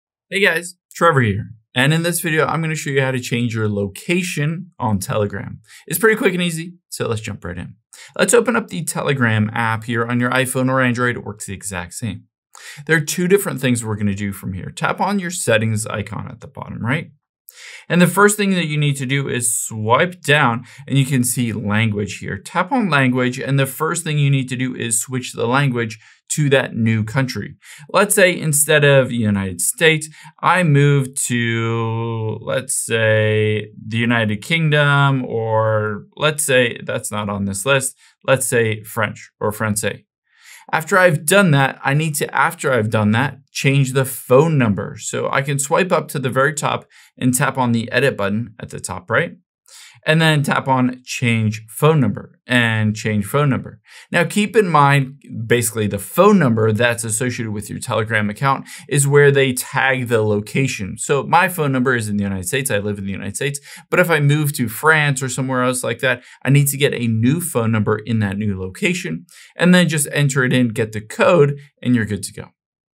How To Change Location In Telegram
Here's how you can change your current location in Telegram if you moved somewhere or live in a different country or region.